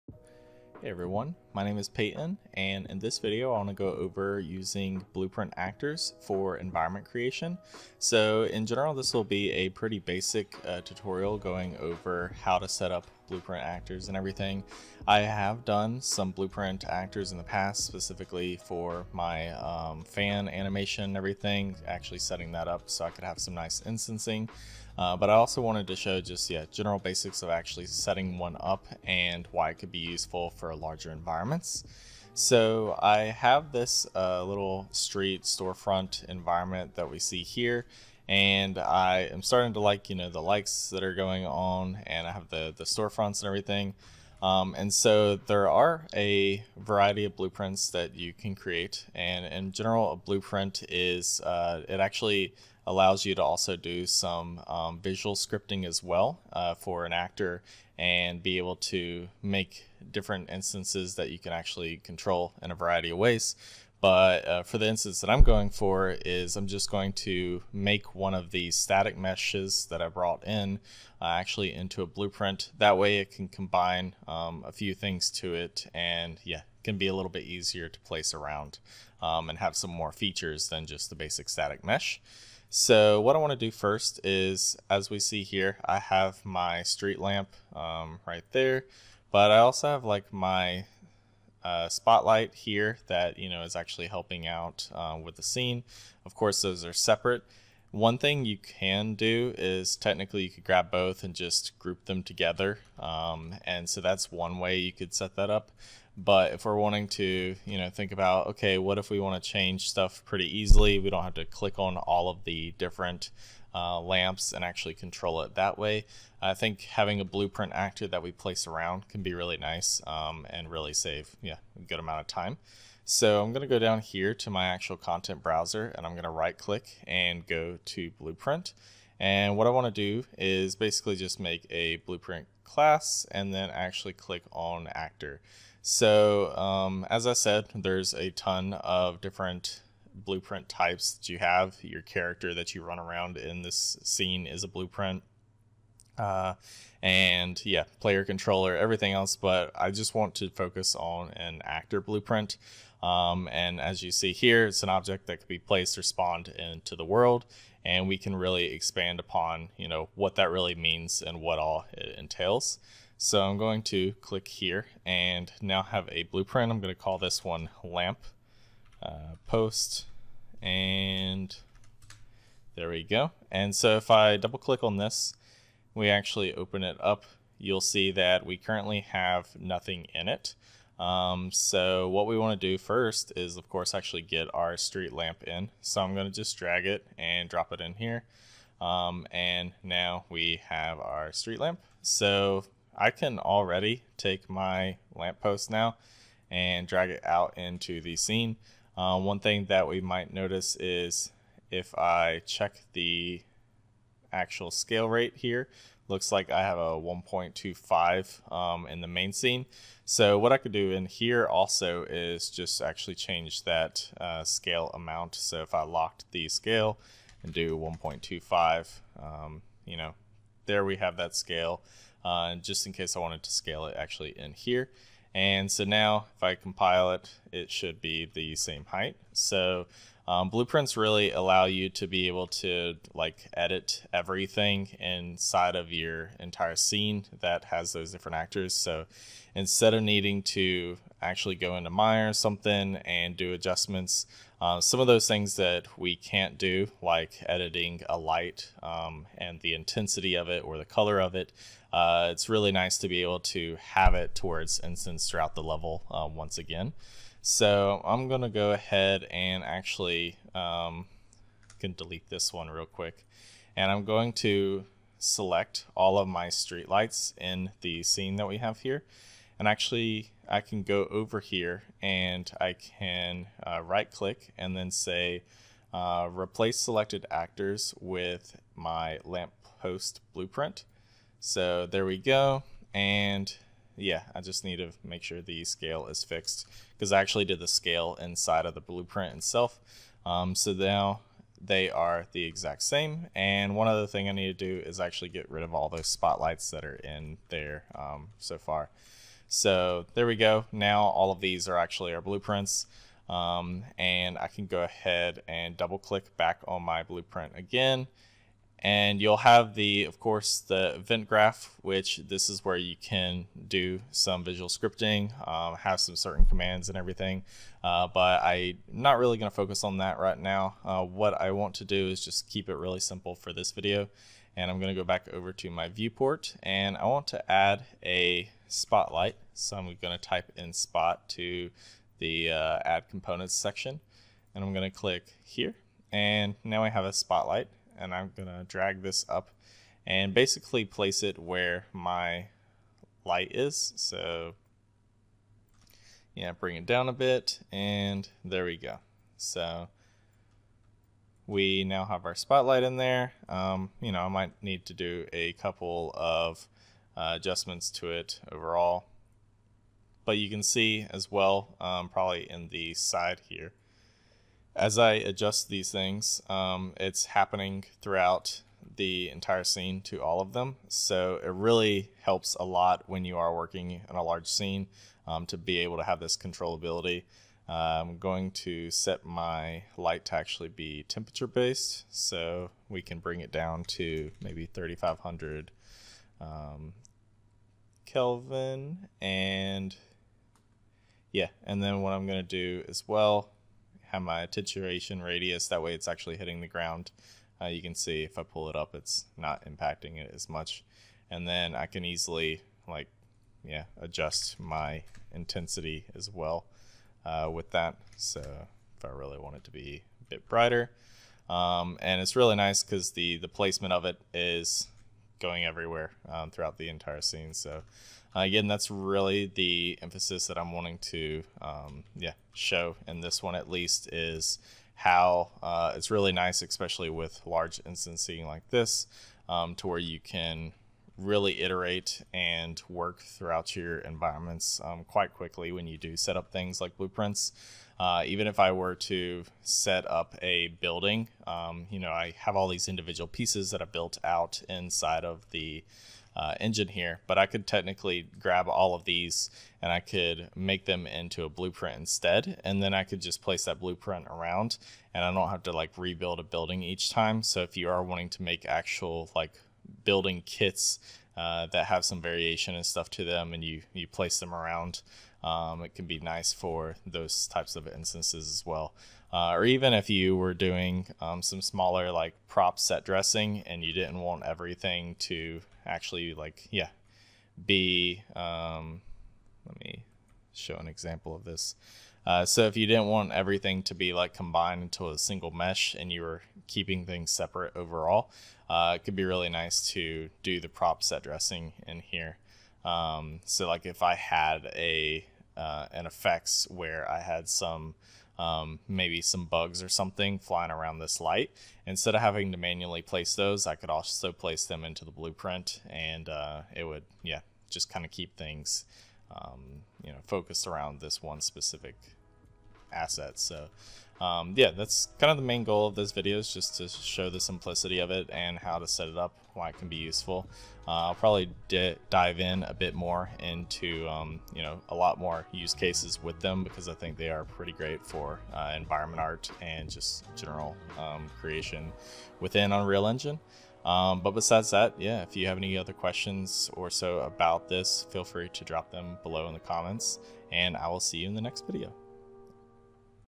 How to Create a Blueprint in Unreal Engine 5 (Beginner Basics)
This video covers the basics of creating an actor blueprint and their usefulness with environment creation. With the limited control of static mesh instances, you rely on whatever you can accomplish from an outside modeling program. Using blueprints to make an instance inside of the engine allows you to add a lot more and have more controllability throughout your scenes.

Hope you found this helpful and let me know down below if you have any questions or topics that you would like covered in future videos!

or here directly on the channel!